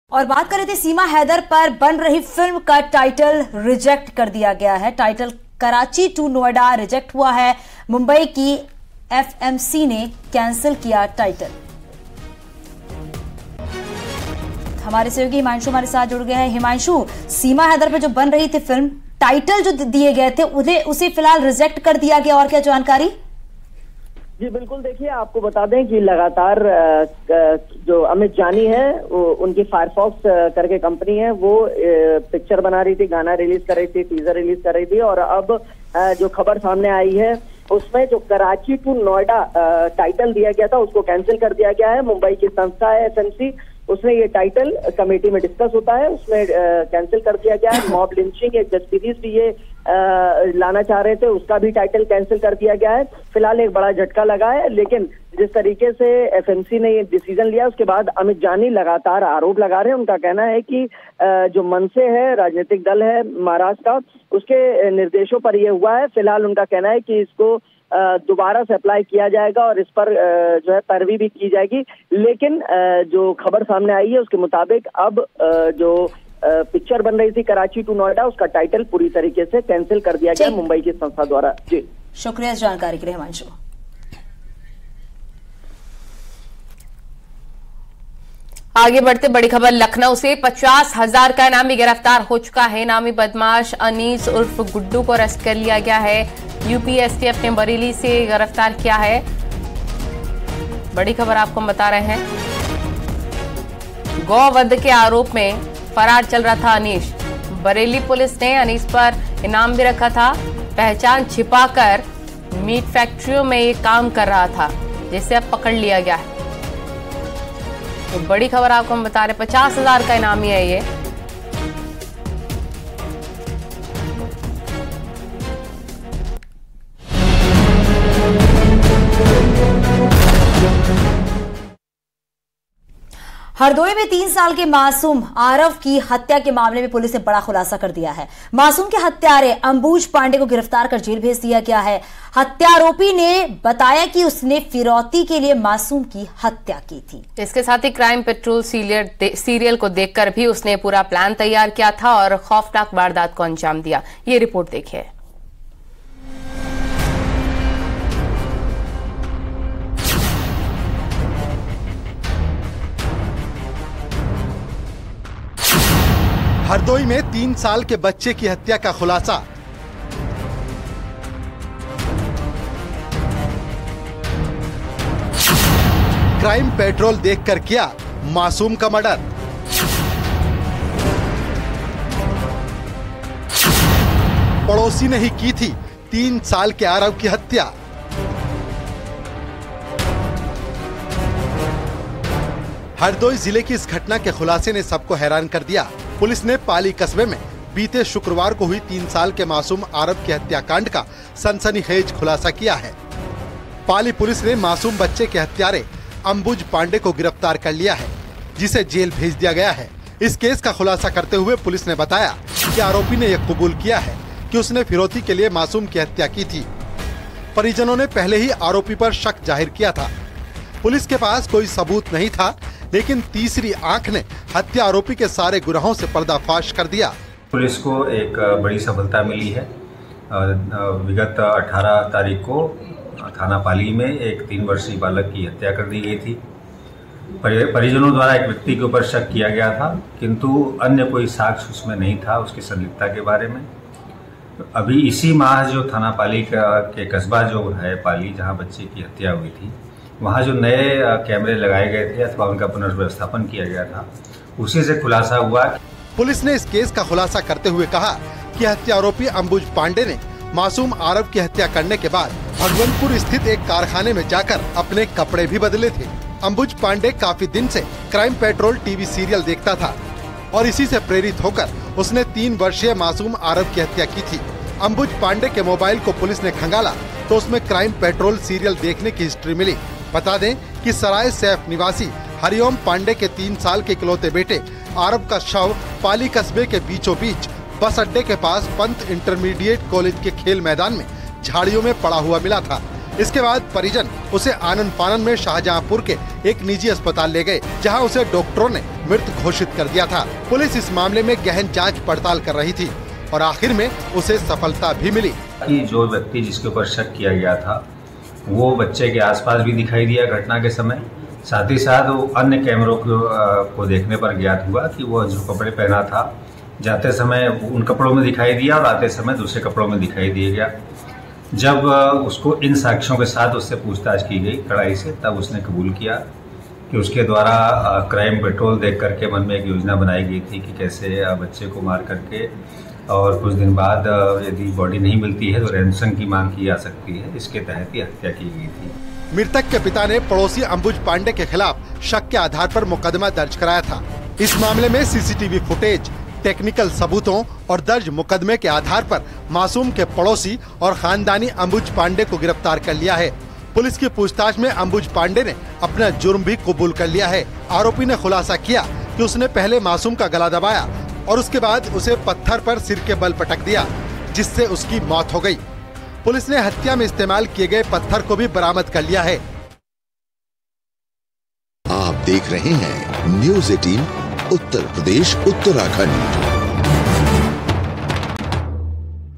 Seema Haidar News: सीमा पर बन रही फिल्म का टाइटल क्यों हुआ रिजेक्ट? | Karachi to Pakistan। Sachin
पाकिस्तना से भारत आई सीमा हैदर पर बन रही फिल्म का टाइटल रिजेक्ट कर दिया गया है, बता दे कि मुंबई की FMC ने कैंसल किया है टाइटल, देखिए पूरी खबर